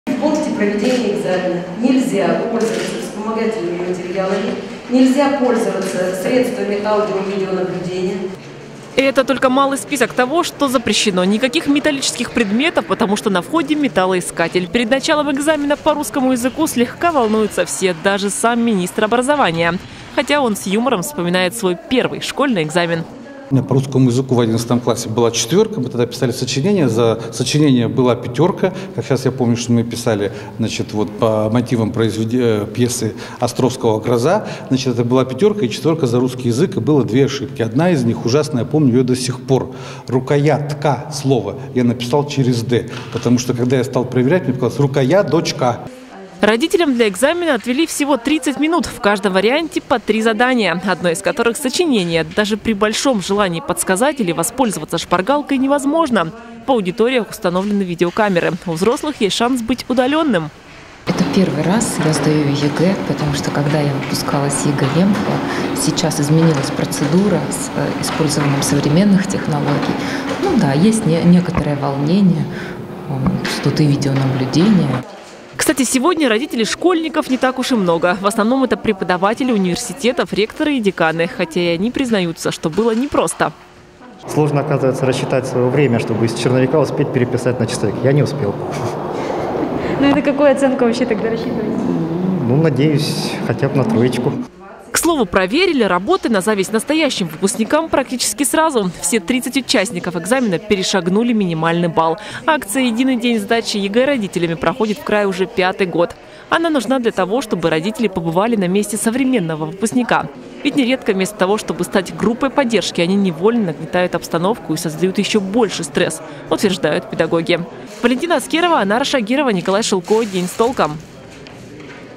Родители выпускников сдавали ЕГЭ по русскому языку
А вот 22-ю гимназию Барнаула сообщение с угрозами, к счастью, миновало. Сегодня там провели ЕГЭ для родителей школьников. Причем всё было по-настоящему – видеонаблюдение, металлоискатели и жесткий регламент. Наша съемочная группа побывала на экзамене, и оценила его сложность для испытуемых.